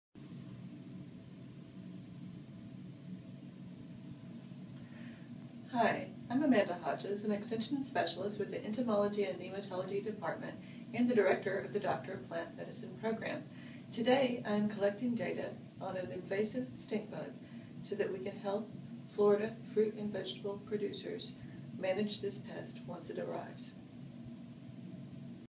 Dr. Amanda Hodges | Associate Extension Scientist; DPM Program Director
Dr. Amanda Hodges is director of the UF/IFAS Doctor of Plant Medicine program and an extension scientist in the Entomology and Nematology Department. She is conducting trials on an invasive pest so she can help protect Florida fruit and vegetable production. Learn more about UF/IFAS study of invasive species